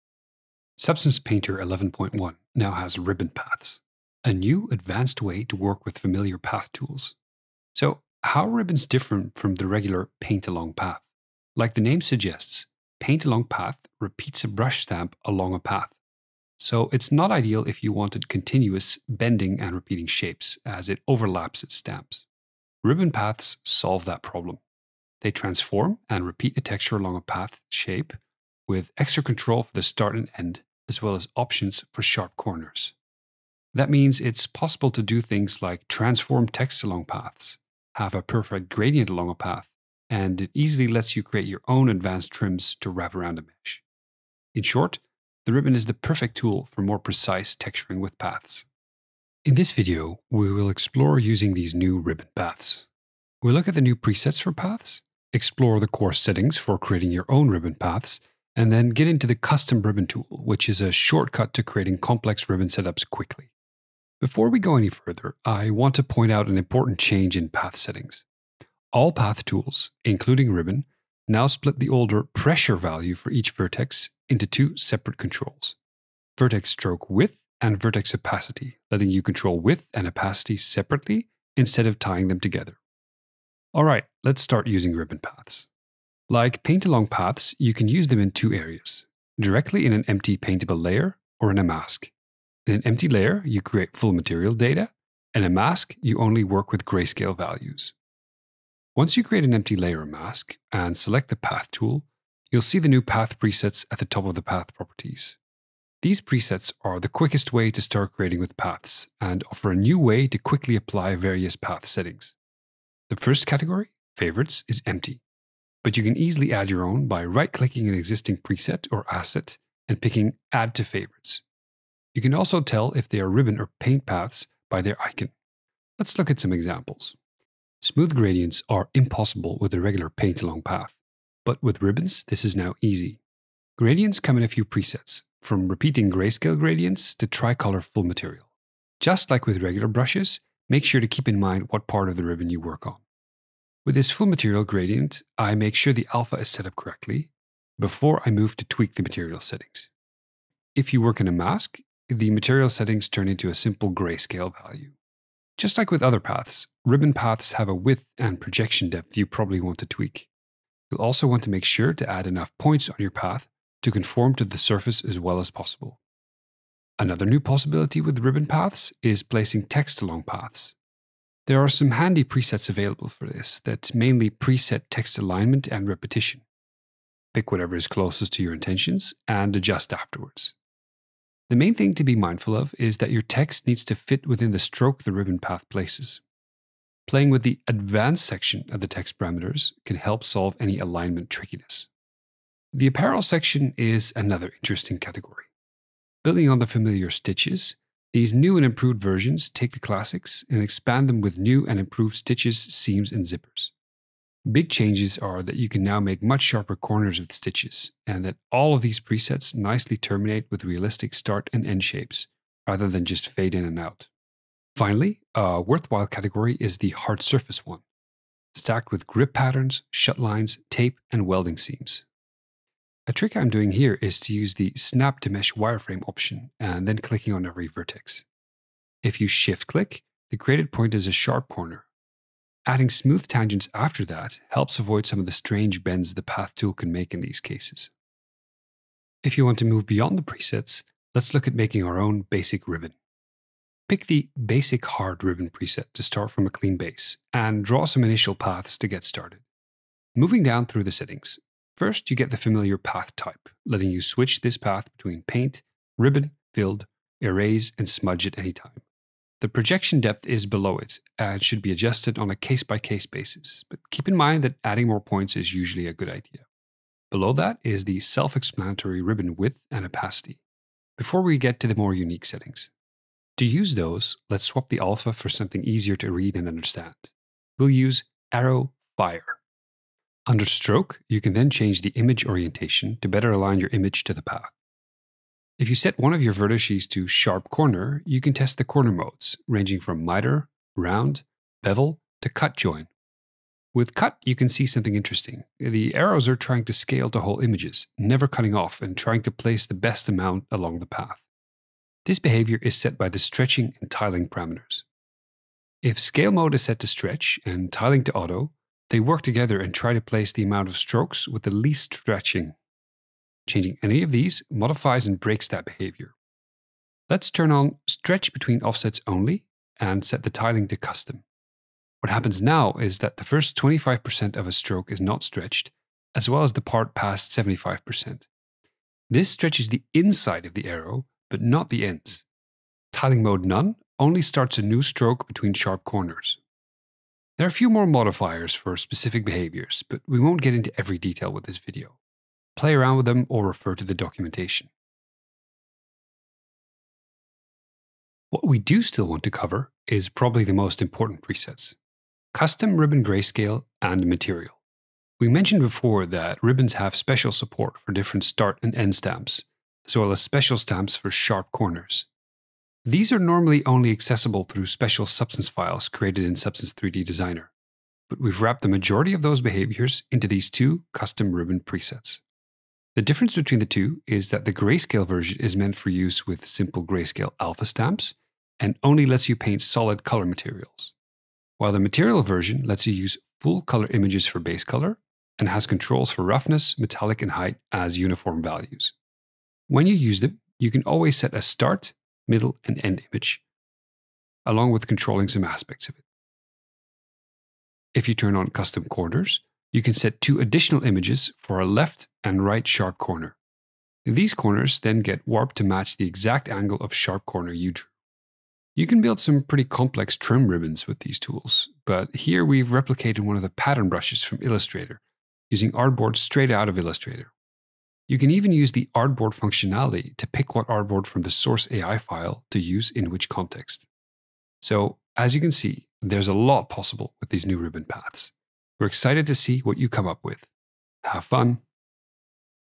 New Ribbon Paths in Substance 3D Painter | Adobe Substance 3D
Substance 3D Painter 11.1 now has Ribbon paths, a new way to work with familiar path tools.

In this video, we explore the use of these new Ribbon paths. We’ll look at the new presets for paths, explore the core settings for creating your own ribbon paths, and then get into the Custom Ribbon tool

Timestamps:
00:27 Intro
01:04 Vertex Stroke Width/Opacity
01:24 Empty Layer/Mask
01:53 Favourite Paths
02:08 Gradients
02:46 Text Along Paths
03:13 Apparel Presets
03:34 Hardsurface Details
03:59 Make A Basic Ribbon
06:12 Grayscale Vs. Material
06:29 Customize Start/End
06:49 Adobe Illustrator Artboards